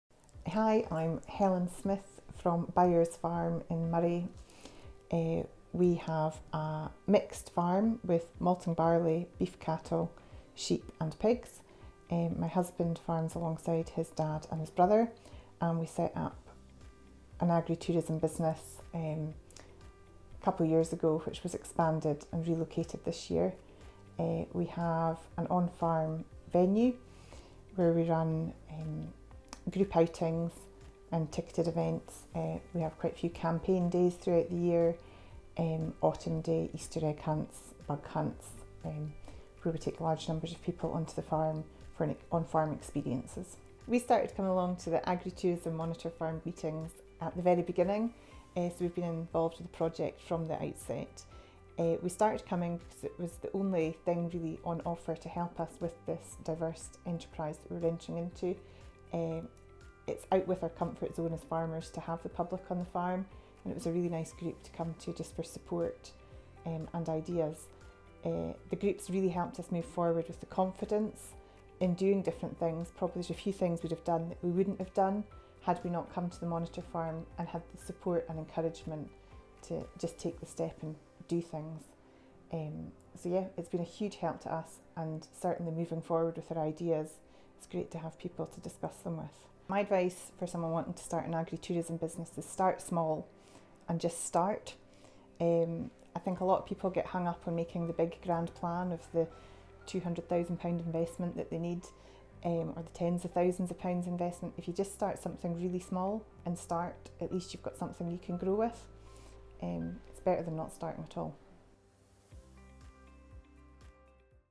Agritourism Monitor Farm | COMMUNITY GROUPS | Byres Farm
Byres farm is a mixed arable and livestock unit on the banks of the River Spey in Moray. It is farmed by the Smith family, who have created a unique facility on the farm to allow children and the public to see first hand what happens on a real farm.

Helen and Bill Smith from Byres farm are regular attendees at the Scottish Agritourism Monitor Farm meetings and have found the project to be a great benefit to developing their business, giving them the opportunity to network with likeminded people.